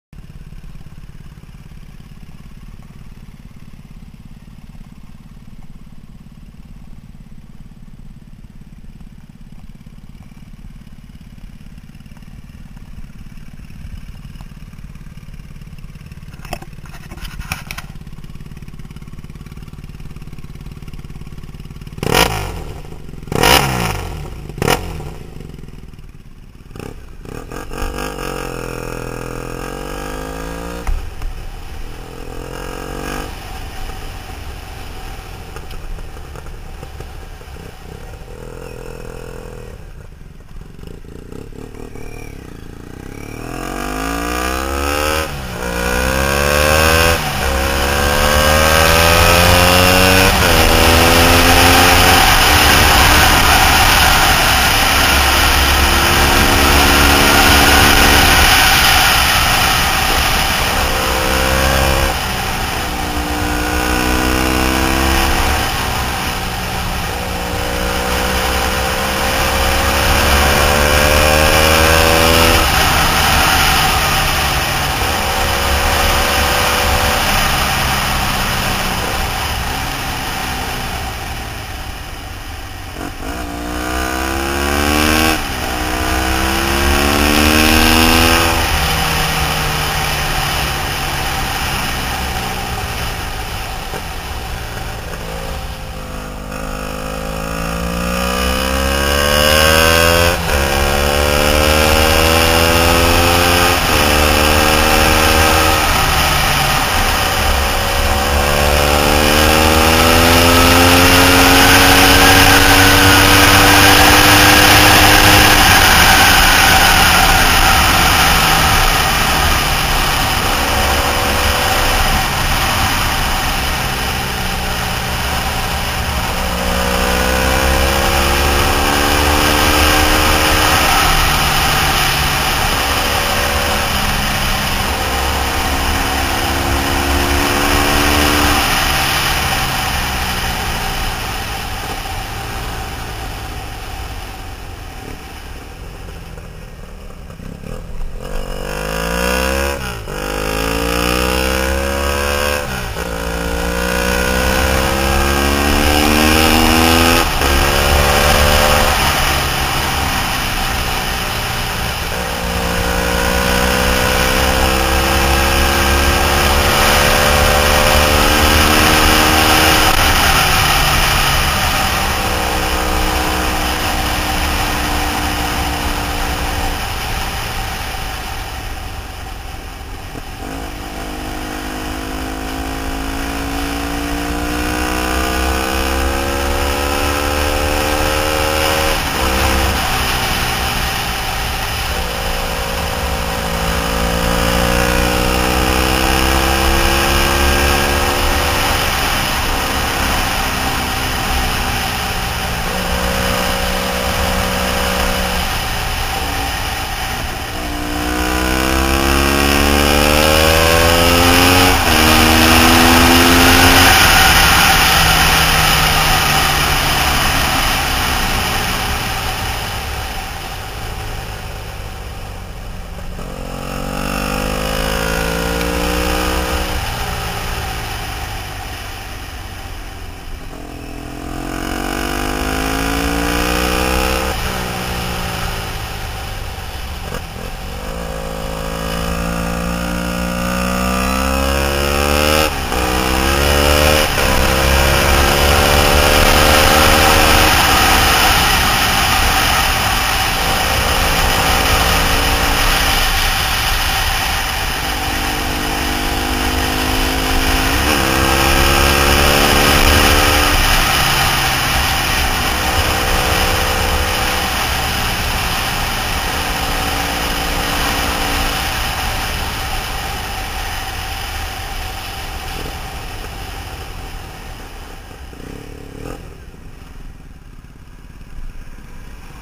Triumph Thruxton ride D&D 2 into 1 exhaust
Quick ride on my Triumph Thruxton 900 cafe racer with D&D pipe, K&N filter, de-snorkeled airbox, jetted carbs, homemade fender eliminator, and Triumph flyscreen and fork gaiters.